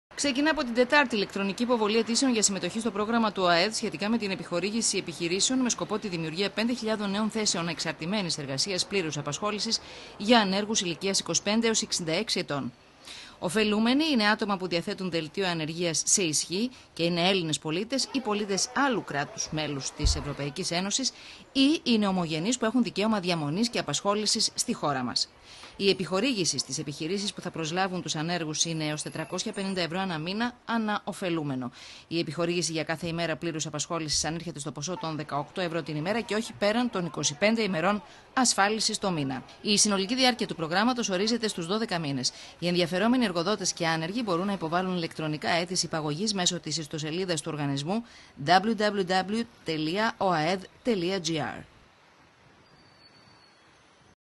ΟΑΕΔ: Από την Τετάρτη οι αιτήσεις συμμετοχής για πρόσληψη 5000 ανέργων
Ξεκινά από την Τετάρτη η ηλεκτρονική υποβολή αιτήσεων για συμμετοχή στο πρόγραμμα του ΟΑΕΔ σχετικά με την επιχορήγηση επιχειρήσεων με σκοπό τη δημιουργία 5.000 νέων θέσεων εξαρτημένης εργασίας πλήρους απασχόλησης για ανέργους ηλικίας 25 έως 66 ετών. Ωφελούμενοι είναι άτομα ηλικίας που διαθέτουν δελτίο ανεργίας σε ισχύ, και είναι Έλληνες πολίτες ή πολίτες άλλου κράτους-μέλους της ΕΕ ή είναι ομογενείς που έχουν δικαίωμα διαμονής και απασχόλησης στη χώρα μας. Η επιχορήγηση στις επιχειρήσεις που θα προσλάβουν τους ανέργους είναι έως 450 ευρώ ανά μήνα ανά ωφελούμενο. Η επιχορήγηση για κάθε ημέρα πλήρους απασχόλησης ανέρχεται στο ποσό των 18 ευρώ την ημέρα και όχι πέραν των 25 ημερών ασφάλισης το μήνα. Η συνολική διάρκεια του προγράμματος ορίζεται στους δώδεκα μήνες. Οι ενδιαφερόμενοι εργοδότες και άνεργοι μπορούν να υποβάλουν ηλεκτρονικά αίτηση υπαγωγής μέσω της ιστοσελίδας του Οργανισμού